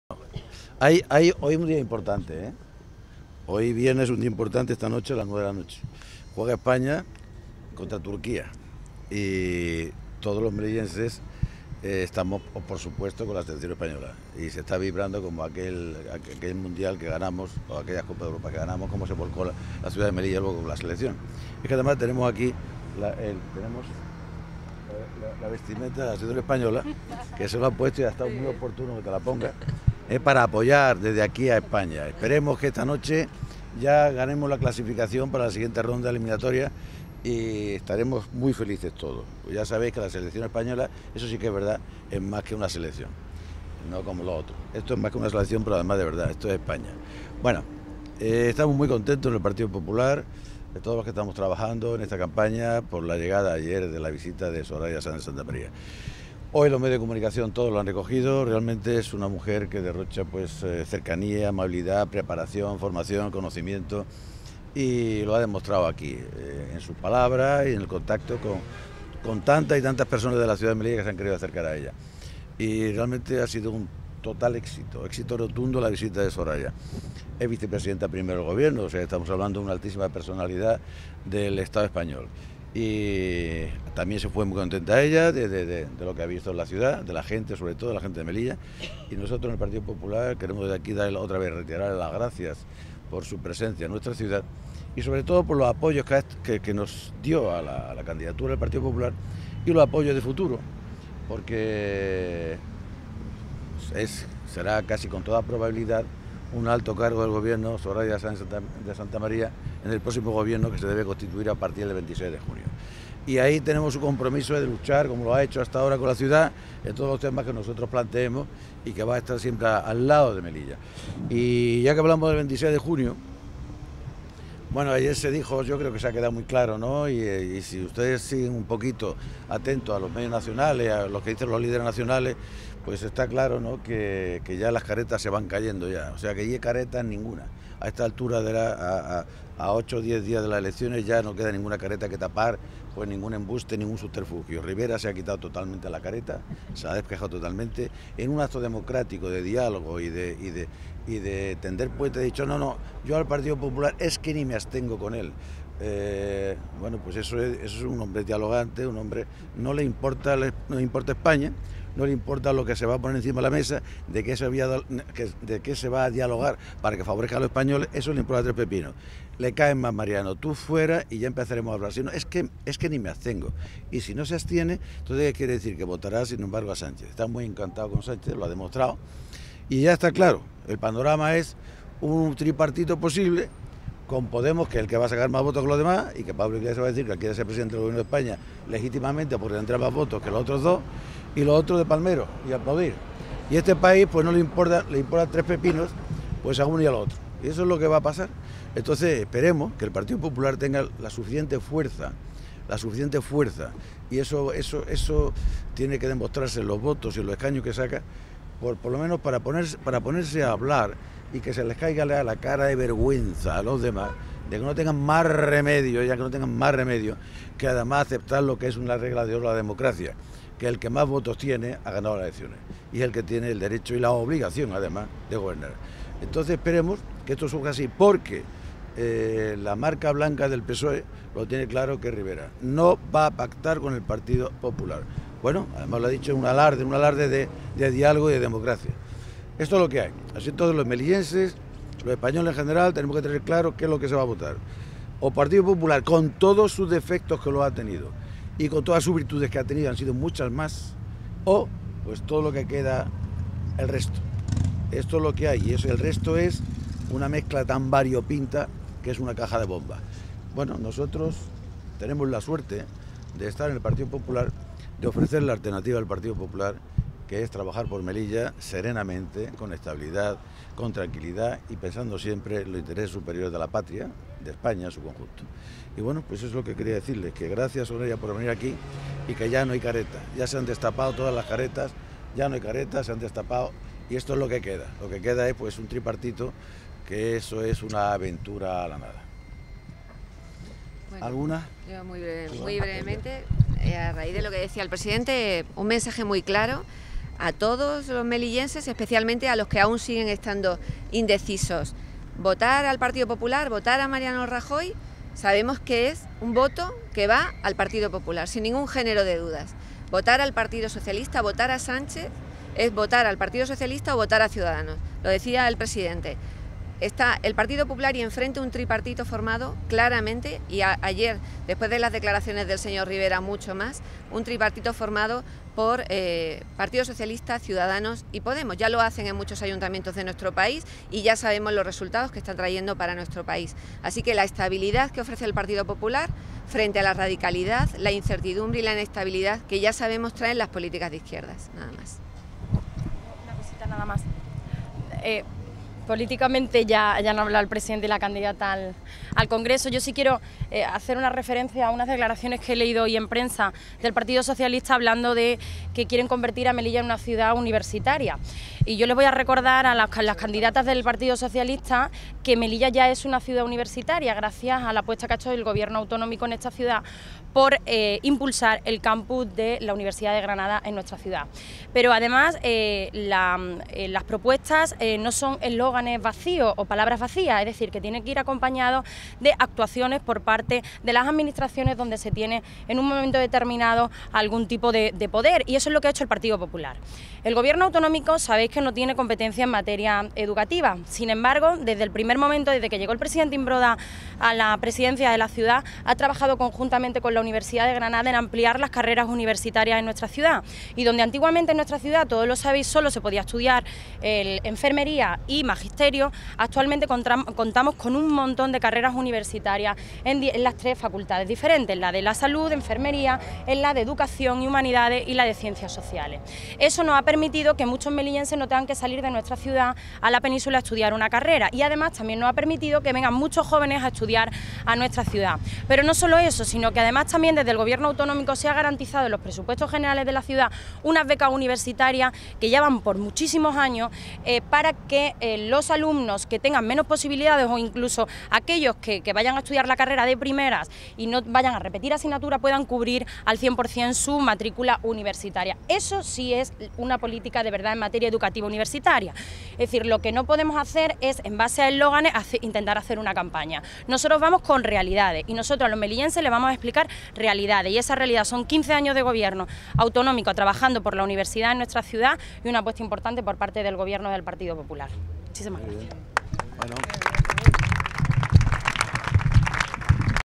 Caravana Popular en Puerto Noray y Paseo Marítimo
Imbroda: “Ciudadanos es la marca blanca del PSOE”

Dueñas: “El voto al PP es para el PP y su candidato”

Acedo recuerda al PSOE que "Melilla ya es una Ciudad Universitaria"